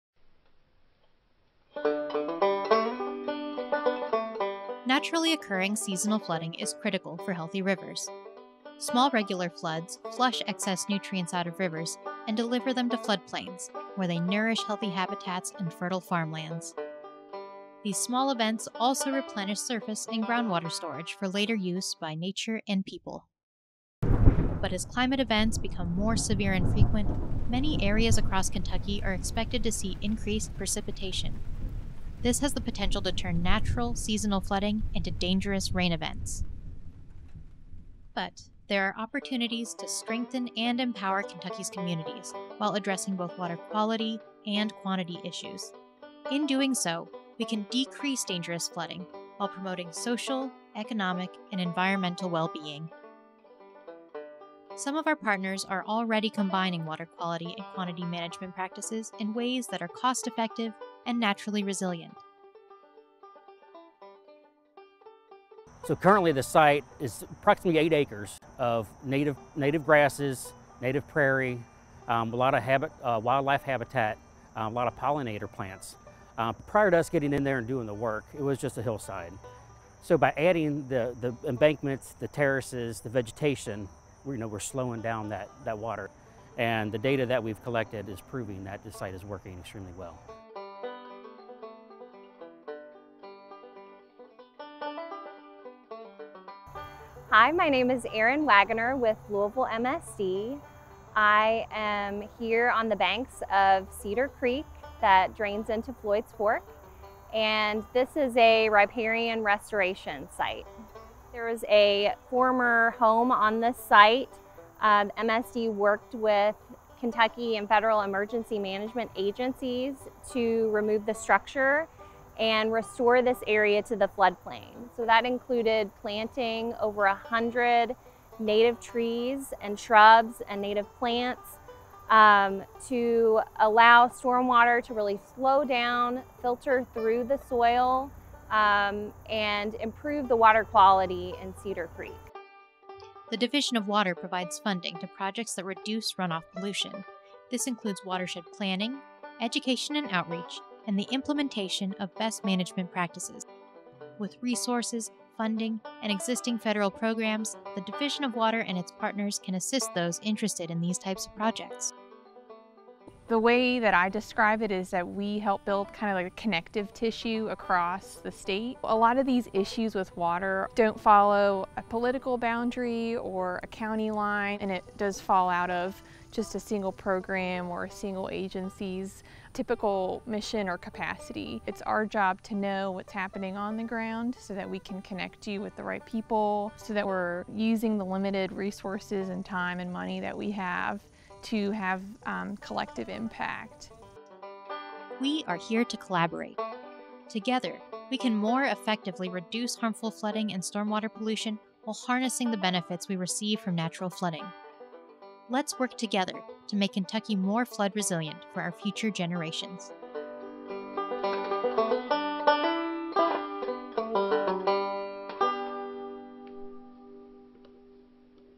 Kentucky Division of Water Nature-Based Solutions
There are opportunities to strengthen and empower Kentucky’s communities, while simultaneously addressing water quality and quantity issues. In doing so, we can mitigate flooding while promoting social, economic and environmental wellbeing.

The Division of Water and its partners can assist community planners, risk managers, and watershed groups interested in practices that mutually address water quality and quantity issues.